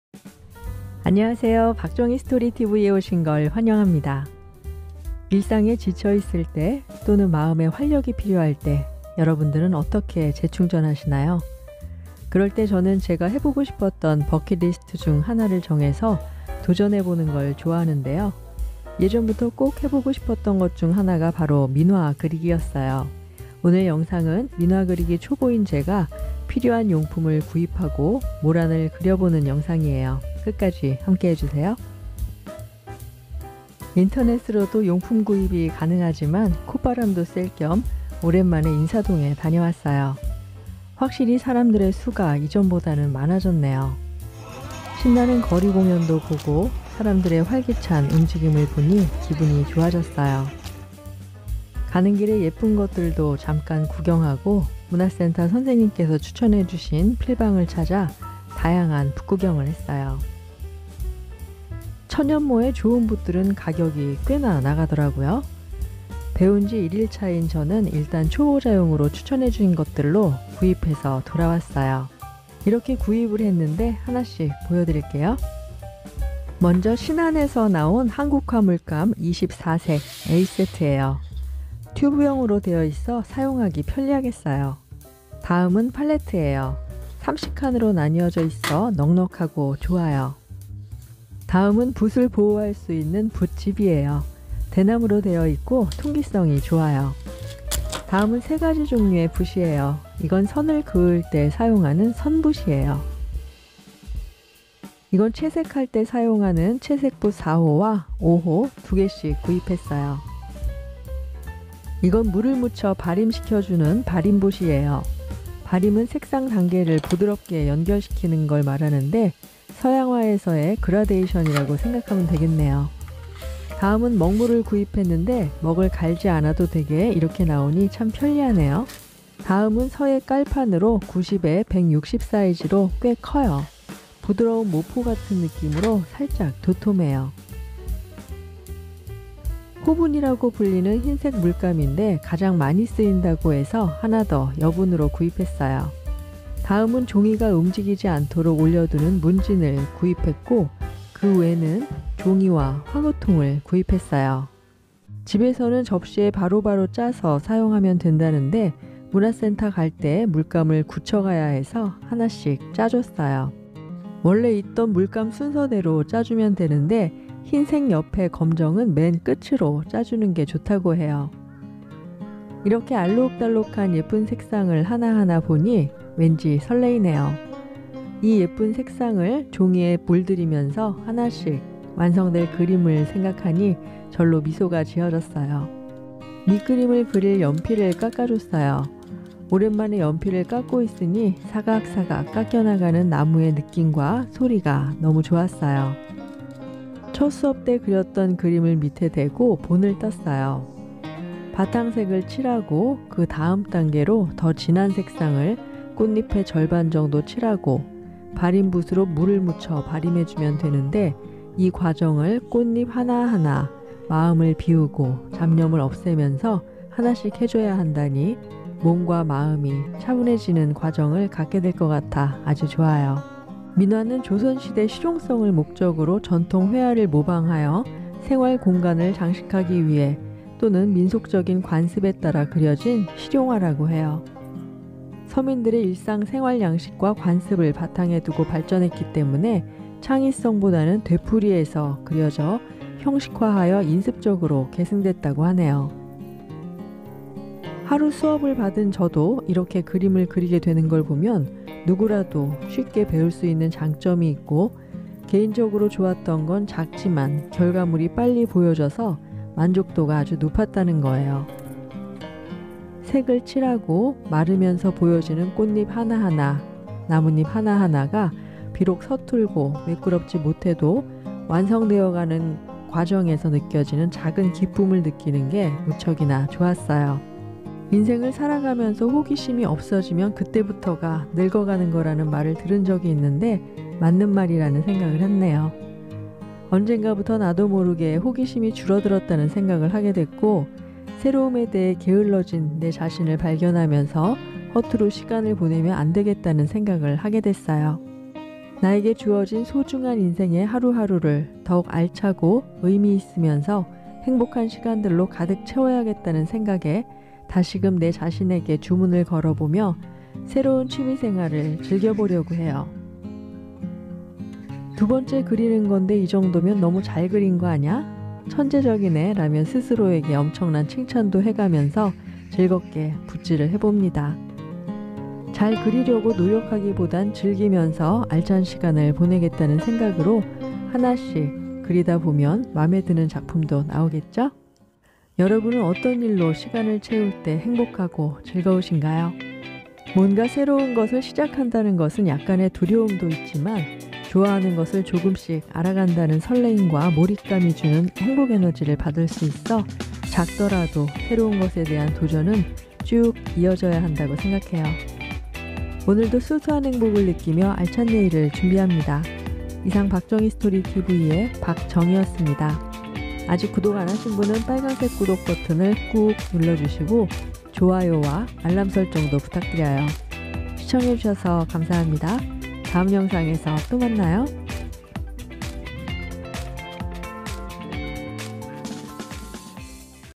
👍50대취미생활추천/정신건강에 좋은 민화그리기/민화초보자의 재료구입/왕초보도 가능한 모란도 그리기
안녕하세요 박정희스토리 tv에 오신걸 환영합니다
일상에 지쳐있을때  또는 마음의 활력이 필요할때 여러분들은 어떻게 재충전하시나요
그럴때 저는 제가 해보고싶었던 버킷리스트중 하느를 정해서  도전해보는걸 좋아하는데요
예전부터  해보고싶었던것중 하나가 바로 민화그리기였어요
오늘영상은 민화그리기 초보인 제가 필요한 용품을구입하고   모란을 그려보는 영상이에요 끝까지 함께해주세요